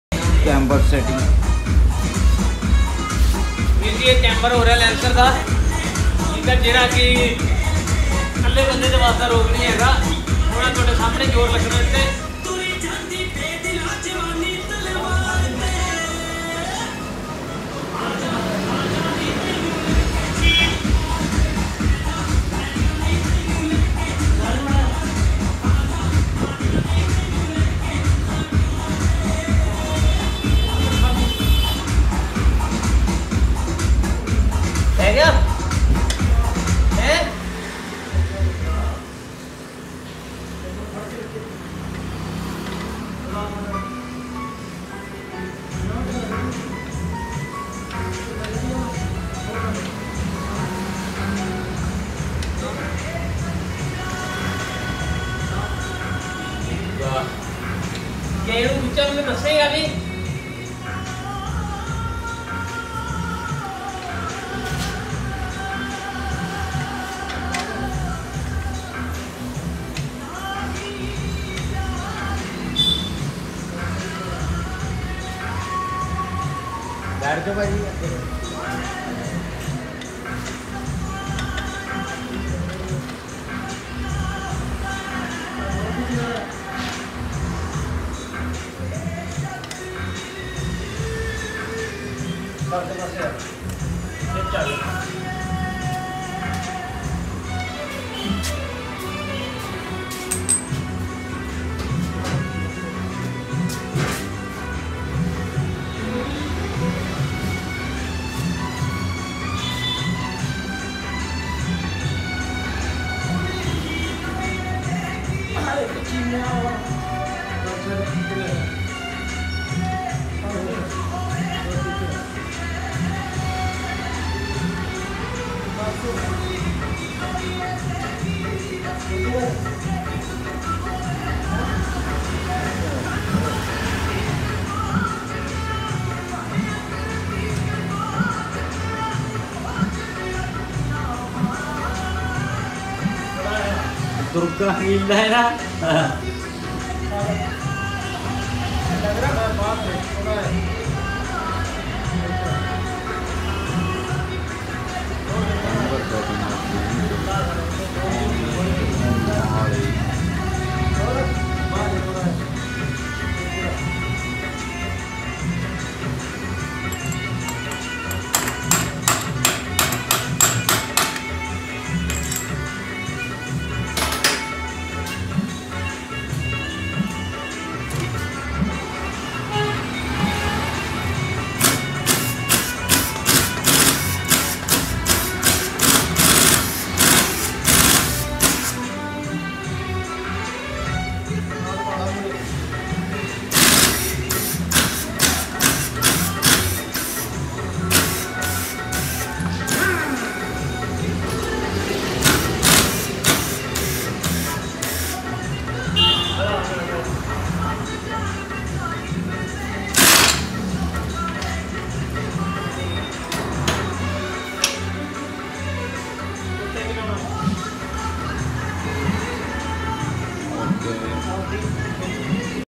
Camber setting
Wheel alignment nd camber setting of lancer .